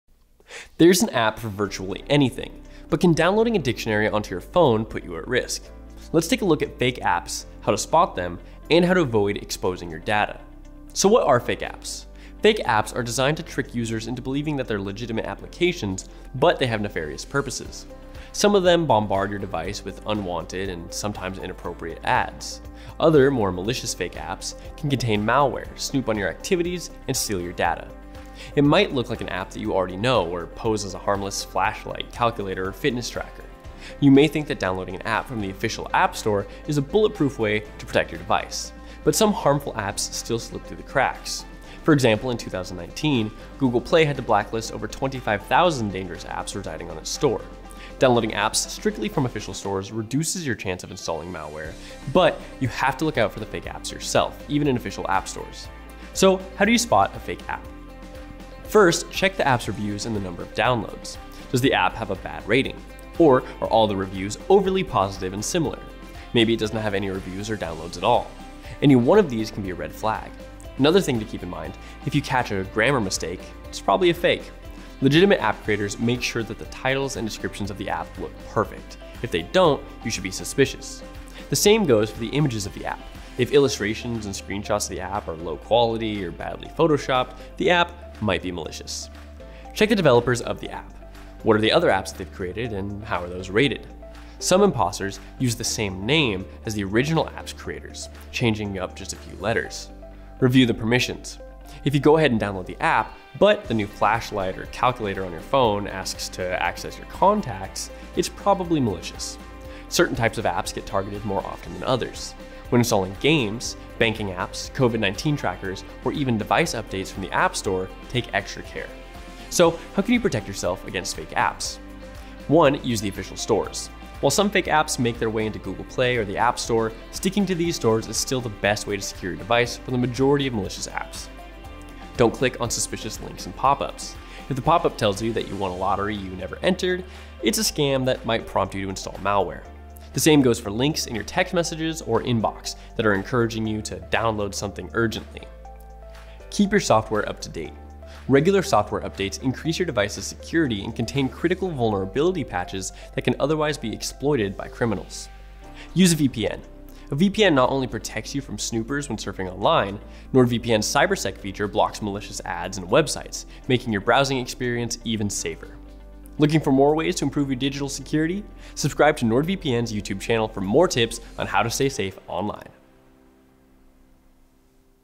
How to identify a fake app | NordVPN
Protect yourself from malware, ads and trackers! 👾
Use our new Threat Protection feature 📣

Now you can get an app for virtually anything, but have you wondered how to identify a fake app? If you're not careful, downloading a harmless-looking calculator or fitness tracker may result in your sensitive data in the hands of criminals. So, what are fake apps? Check out this video to find out that and learn how to protect yourself against fake apps.

Fake apps aren't the only thing that can put your data at risk.

So, what are fake apps? Fake apps are designed to trick the user into thinking they are harmless legitimate applications. However, they are not what they seem. Some of them may track your activities, collect your data, or even spread malware on your device.
But there are ways to identify a fake app and protect yourself against it. Check out our video for tips on how to spot and avoid fake apps.

Protect yourself from malicious websites and ads with NordVPN's CyberSec feature